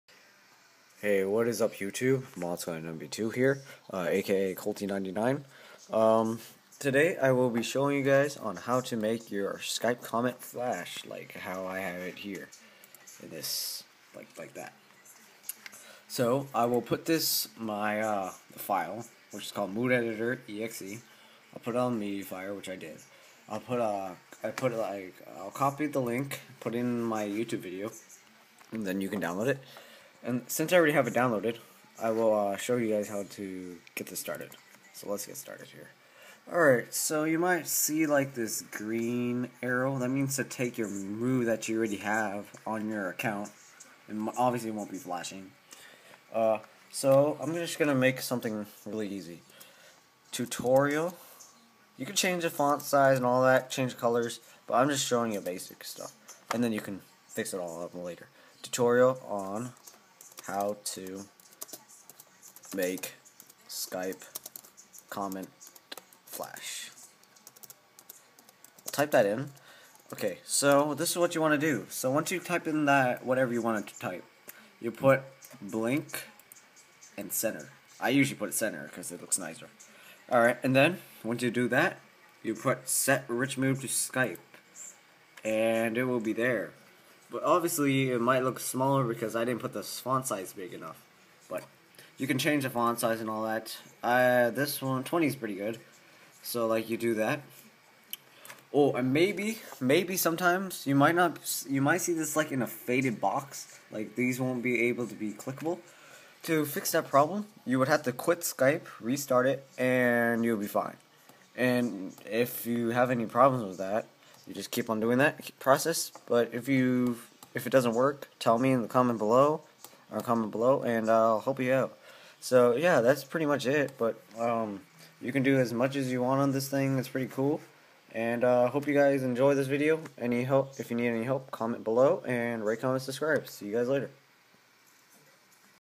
How to:Skype Flashing Comment
In this video I will teach guys on how to make your Skype comment flash. Here is the download link for the application.